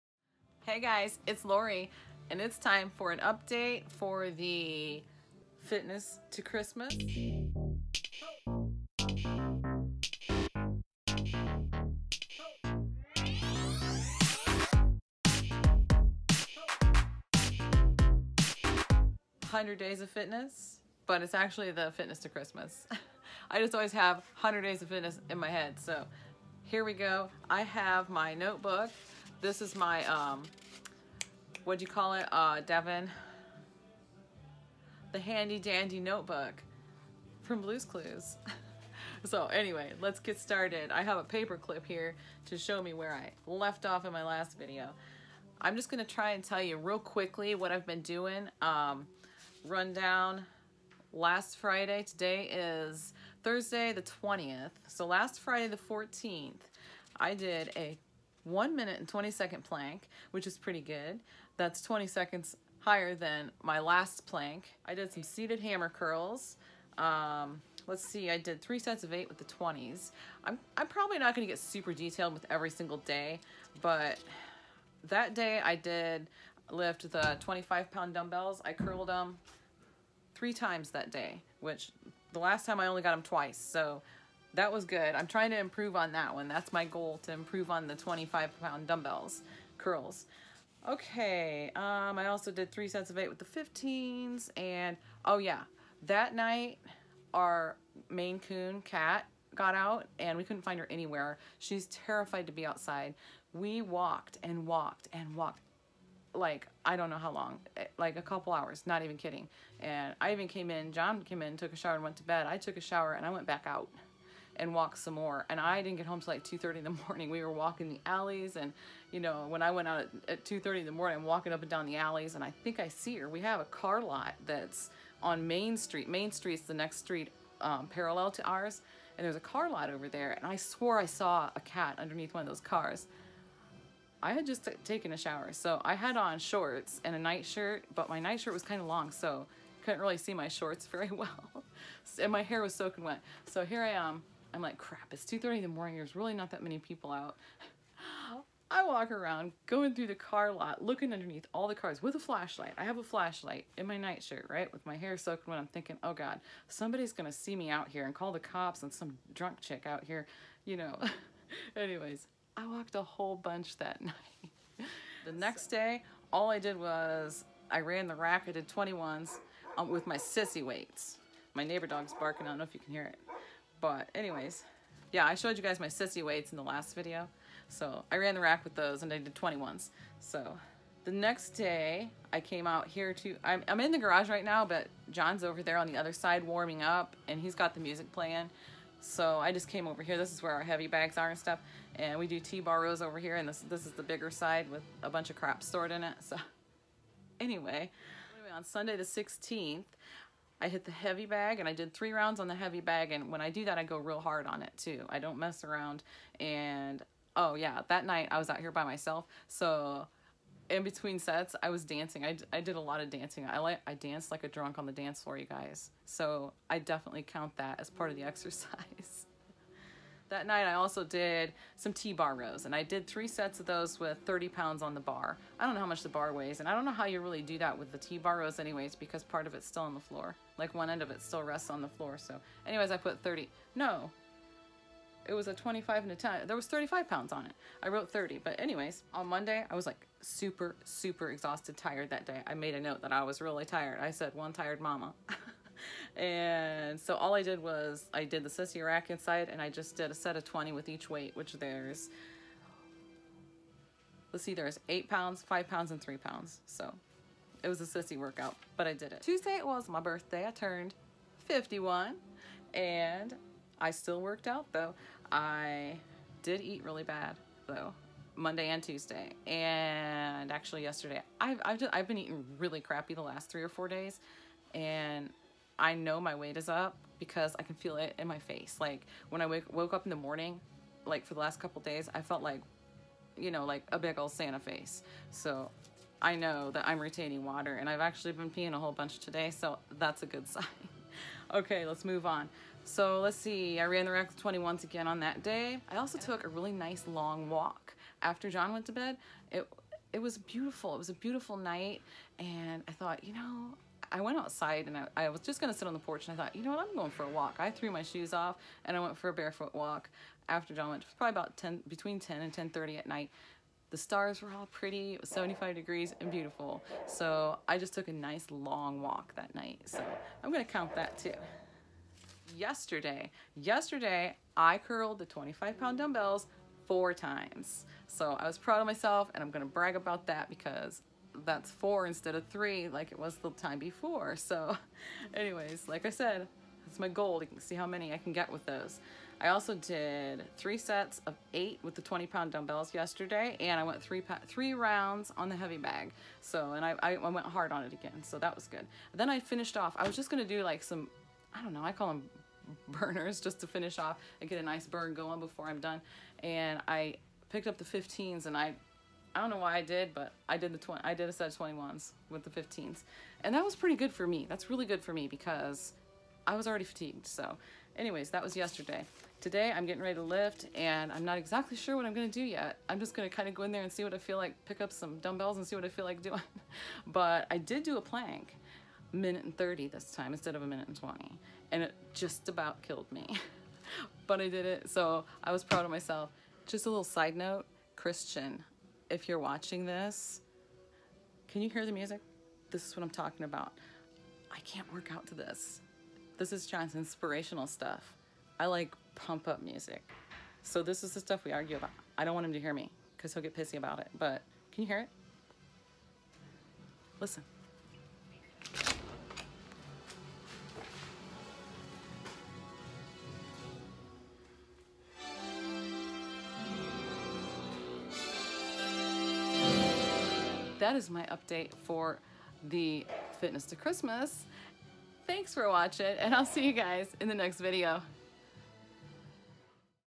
Fitness to Christmas and some Chatter 30 Day Bicep Challenge 100 Days of Fitness
Fitness Update & toward the end,  a Side Note to Christian Durbin... go check out his channel:

Please Support if you like my Channel!
Click Like if you enjoyed this video!